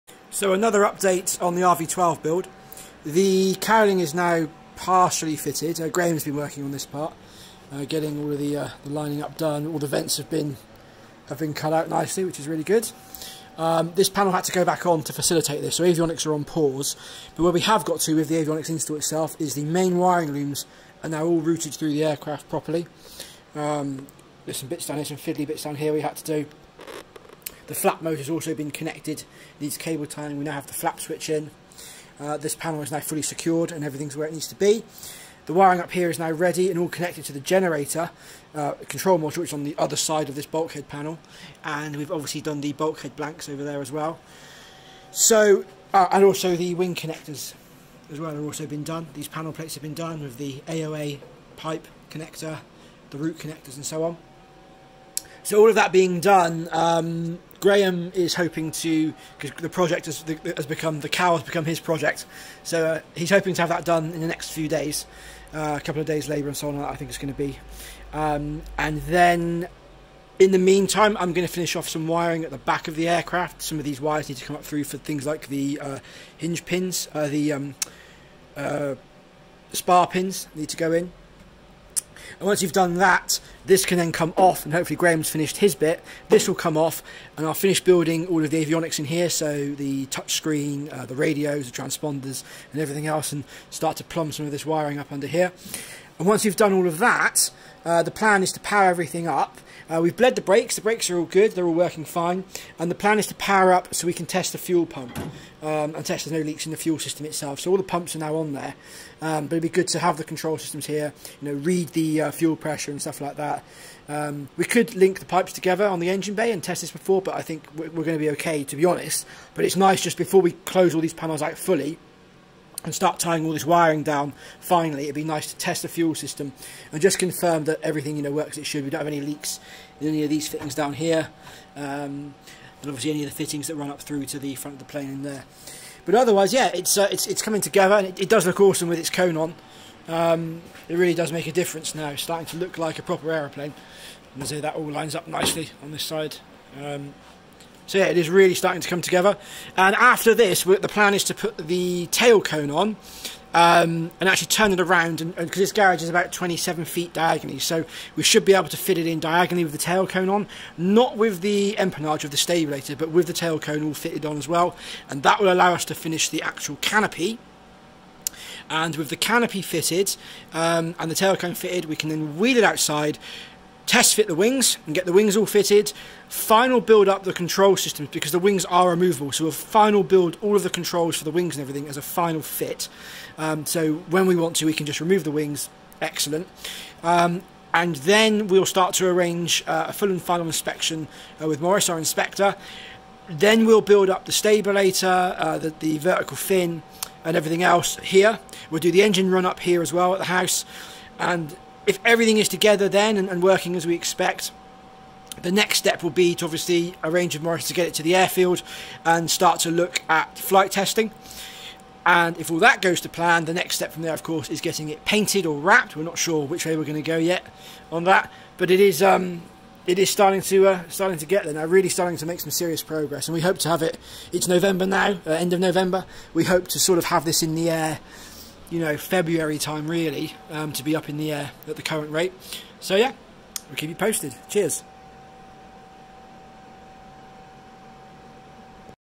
RV-12is Build | Quick Update #4 | The cowling!
So the cowling is all going together now, and the avionics have really progressed!